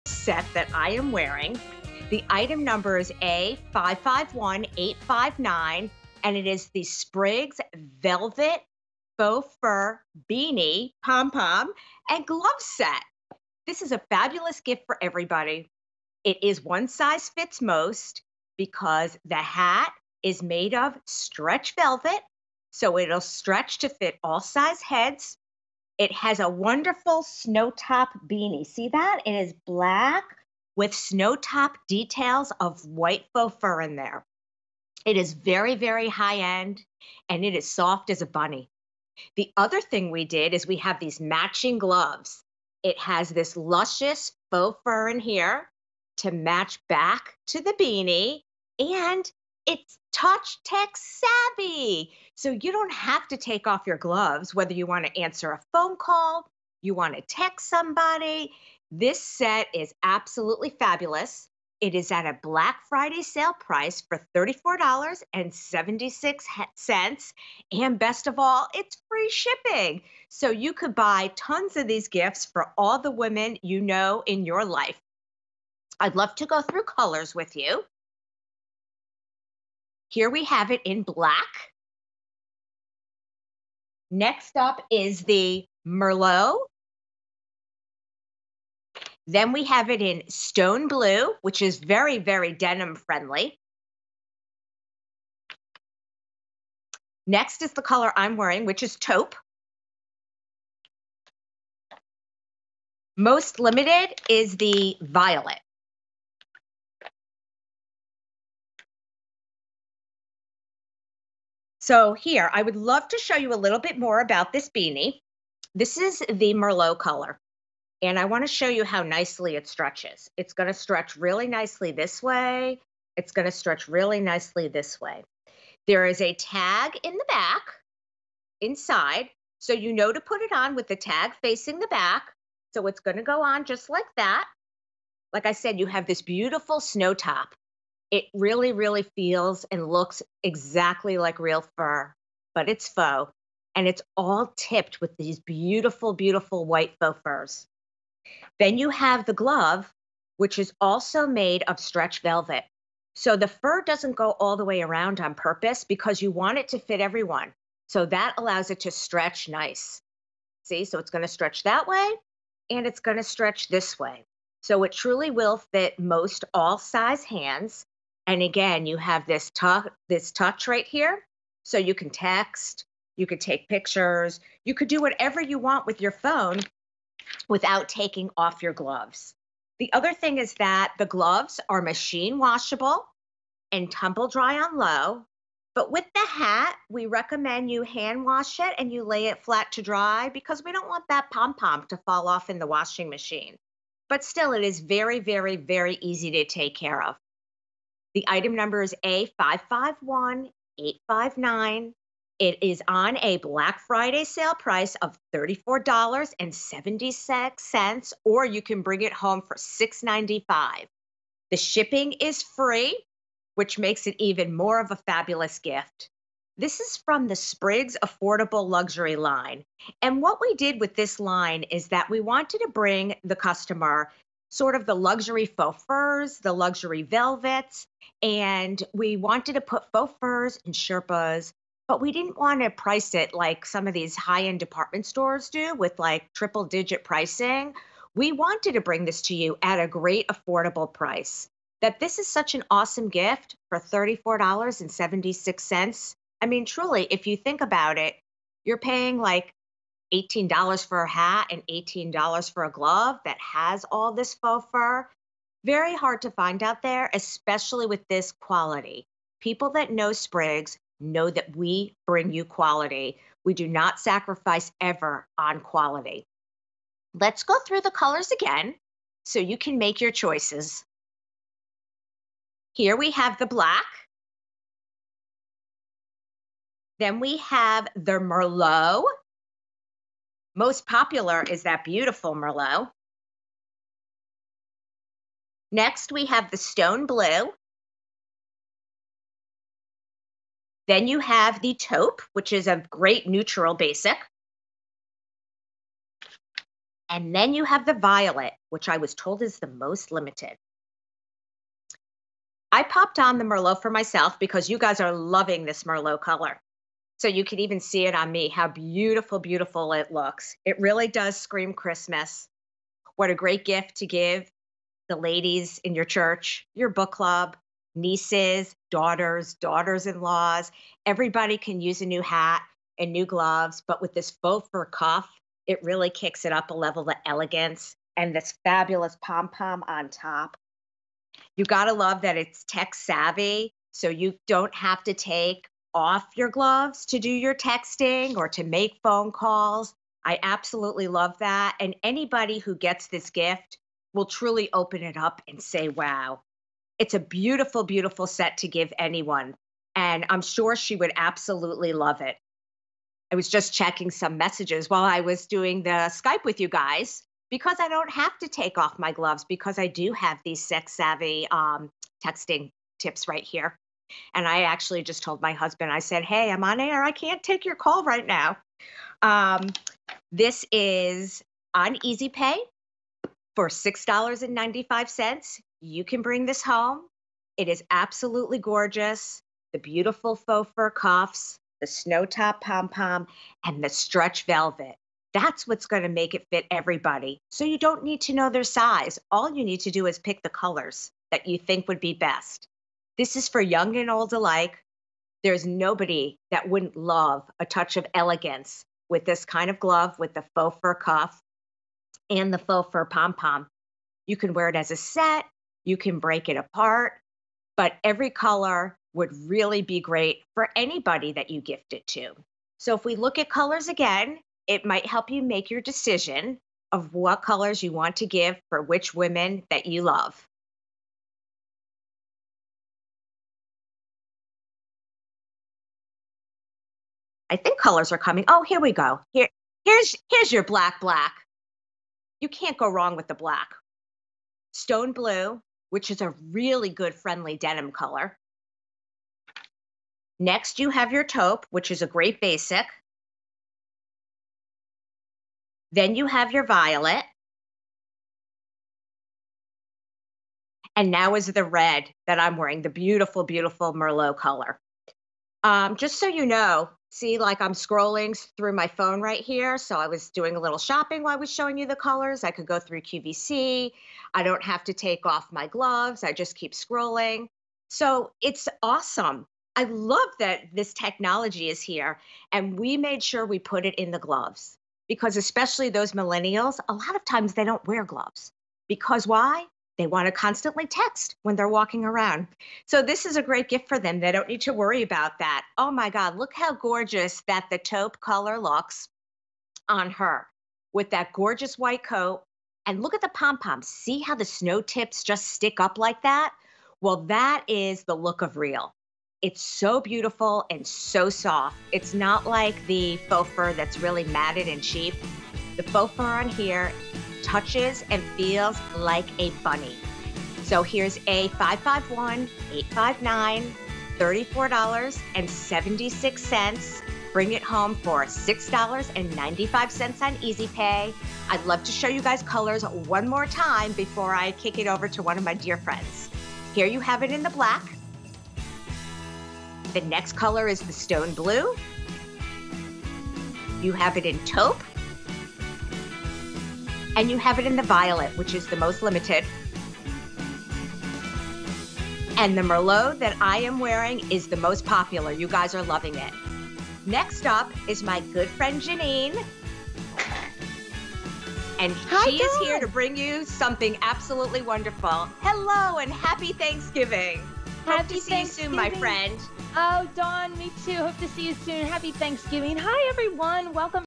Sprigs Velvet and Faux Fur Beanie & Glove Set on QVC
Sprigs Velvet and Faux Fur Beanie & Glove Set

Step out into the wintry weather in chic style with this velvet and faux fur-trimmed beanie and glove set (featuring touchscreen compatible fingertips for easy texting). From Sprigs.